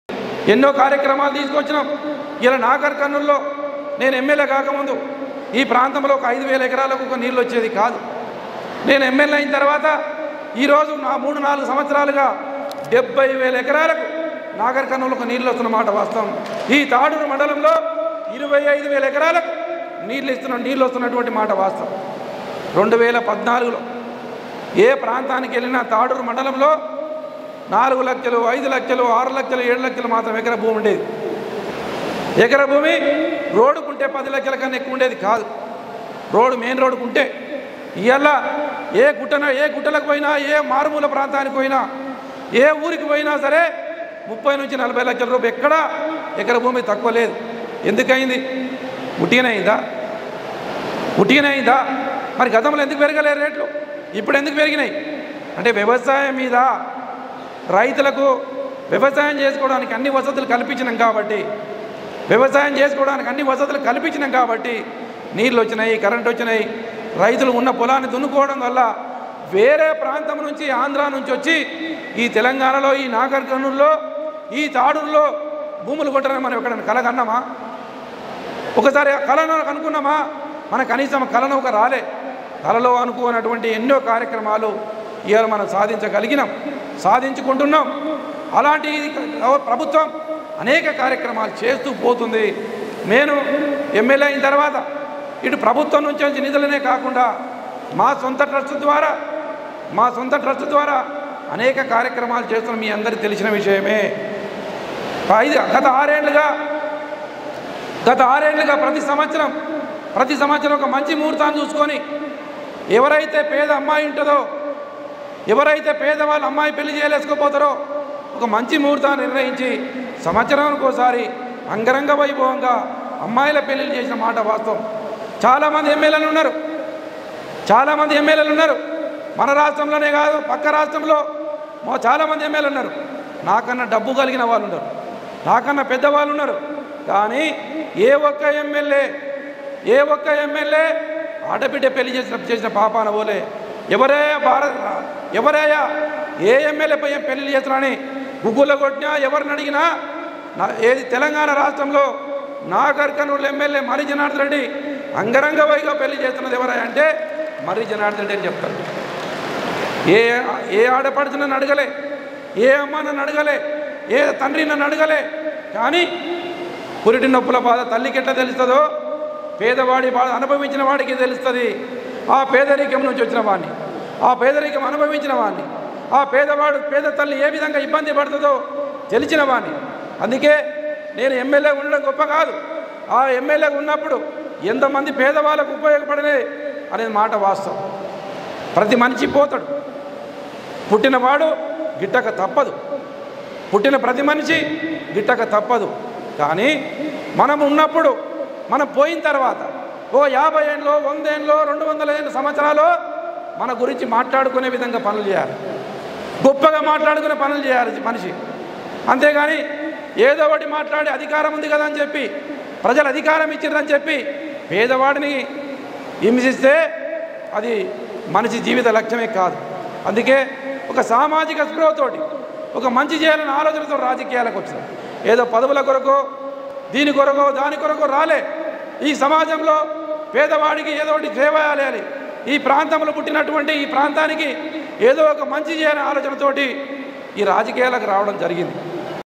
నాగర్ కర్నూల్ ప్రజలకు సేవ చేయడానికే రాజకీయాల్లోకి వచ్చా | MLA Marri Janardhan Reddy | iDream News
MLA Marri Janardhan Reddy | iDream News